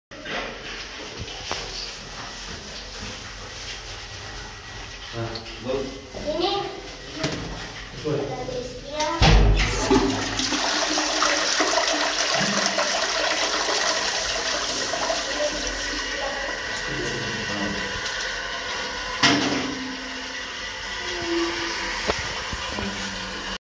2010 Johnson Suisse toilet on a Low Level Johnson Suisse Cistern
This toilet was filmed at a public toilet on Penang Hill (Upper Station) on Penang Hill, Malaysia. This toilet is average. Rating: 7/ 10.